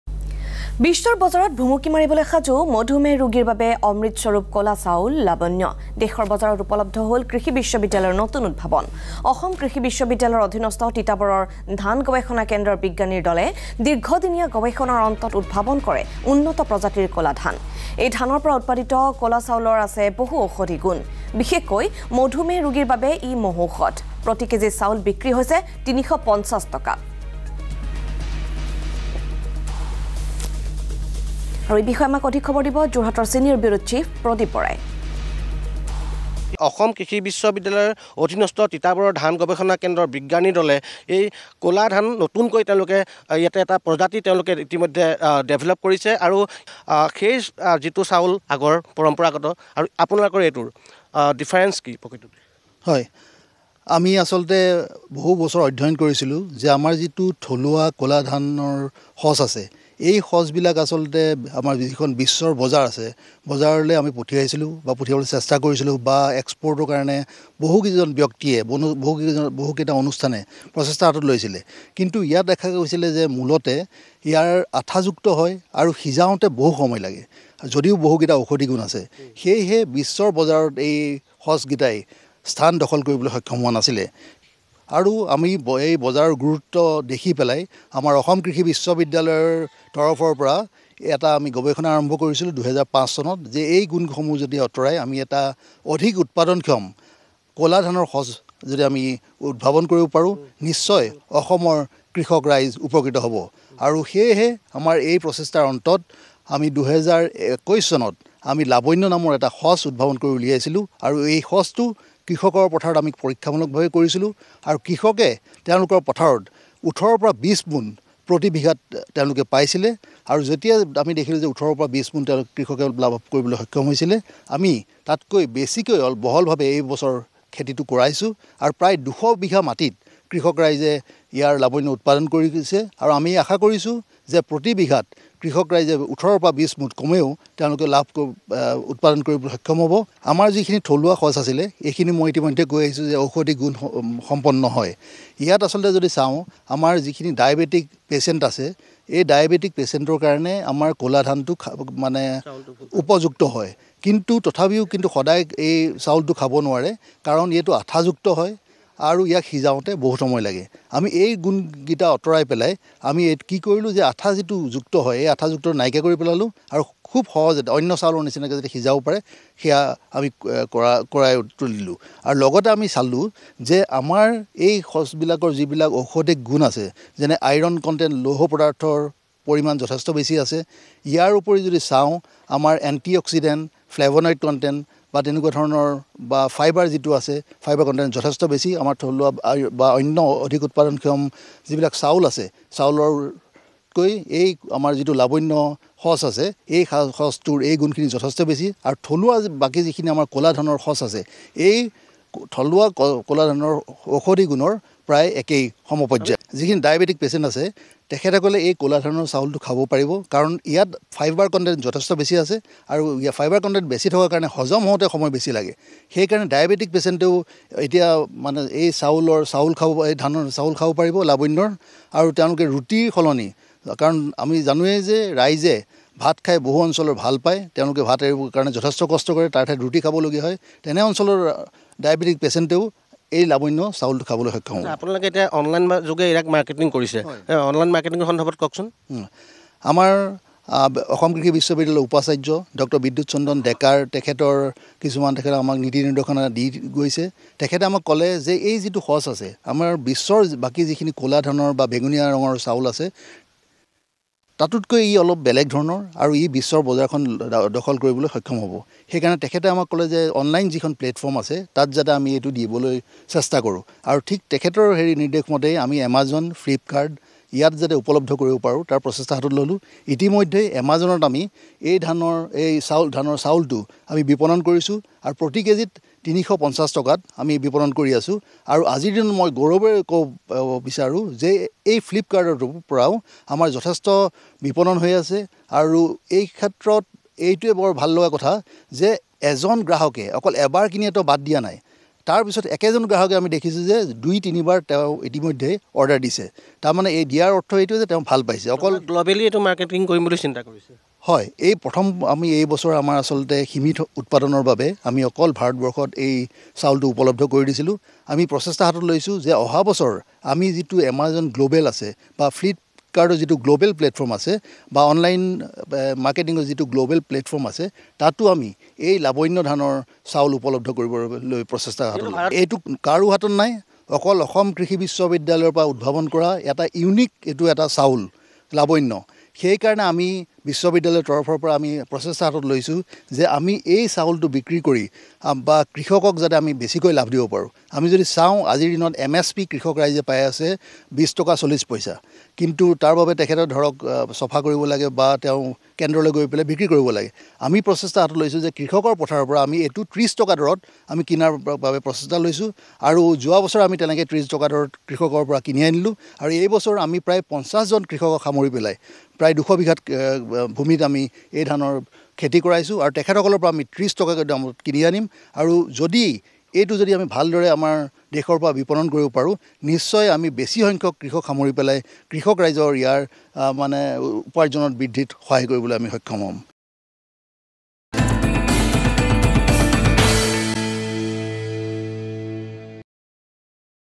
বিশ্বৰ বজাৰত ভুমুকি মাৰিবলৈ সাজু মধুমেহ ৰোগীৰ বাবে অমৃত স্বৰূপ কলা চাউল 'লাবন্য'
কৃষি বিশ্ববিদ্যালয়ৰ নতুন উদ্ভাৱন! বিশ্বৰ বজাৰত ভুমুকি মাৰিবলৈ সাজু মধুমেহ ৰোগীৰ বাবে অমৃত স্বৰূপ কলা চাউল 'লাবন্য'। এমাজন, ফ্লিপকাৰ্টৰ জৰিয়তে বজাৰত উপলব্ধ হ'ল এই বিশেষ চাউল